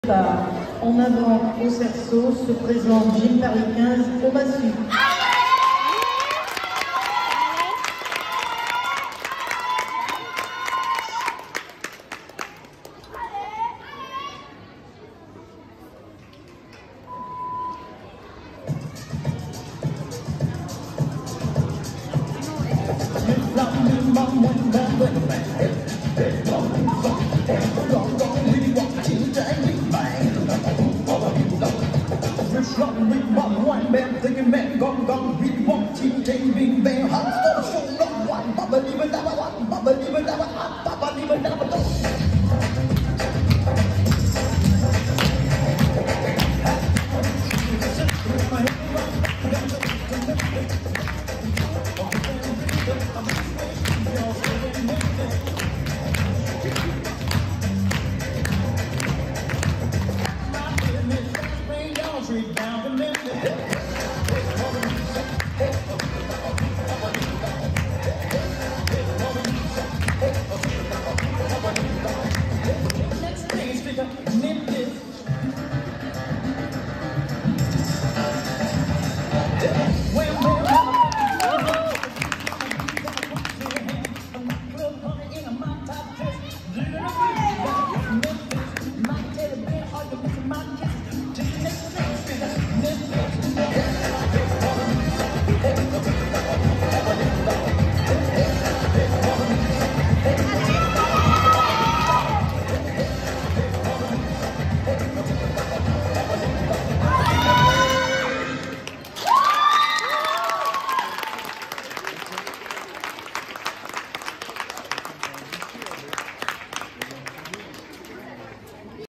Gymnastique Rythmique - Duo aux Massues de Gym' Paris 15
Gymnastique Rythmique
Championnat départemental
11-12 février 2023, Gymnase Suzanne Berlioux, Paris
Duo aux Massues de Gym' Paris 15 - Médailles d'Or